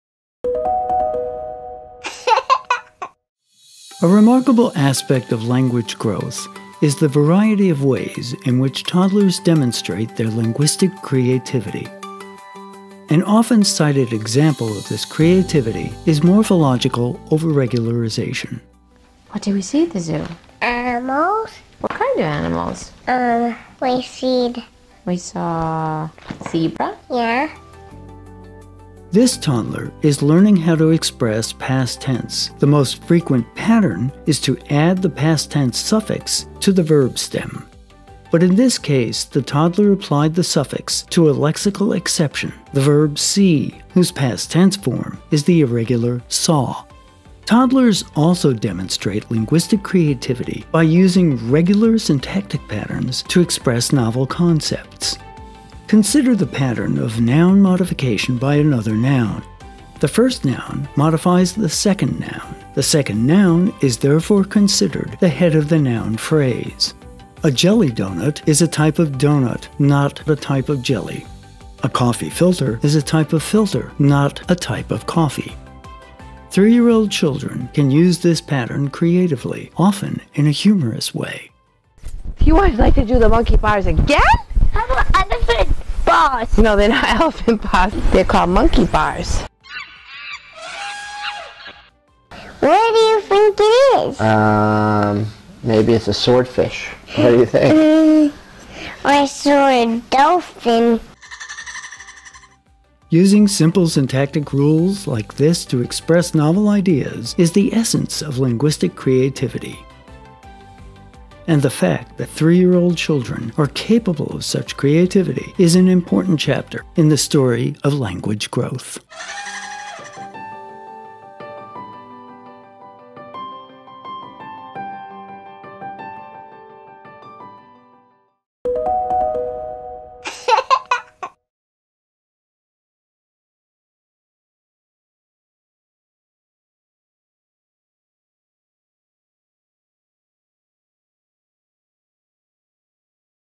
Overregularization and Linguistic Creativity
Toddlers display remarkable creativity with their language. A well-known example of their creativity is verb tense overgeneralization. Overregularization is caused by 'lexical exceptions' to a rule. For example, the verb 'see' has an irregular past tense 'saw', and so toddlers may say 'seed' instead. A much for powerful demonstration of their creativity is found when toddlers use an exceptionless syntactic pattern to express a novel idea. For example, they might create the phrase "elephant bars" on the model of "monkey bars" or "sword dolphin" on the model of "swordfish". This syntactic pattern is called 'noun-noun modification'. The first noun modifies the meaning of the second noun, the second noun being the 'head' of the noun phrase. Clearly, toddlers are using an abstract syntactic pattern to express novel meanings.  This form of linguistic creativity is not due to a mere ‘lexical exception’, but is rather an expression of the way speakers of a language use the rules of their language to create new linguistic expressions.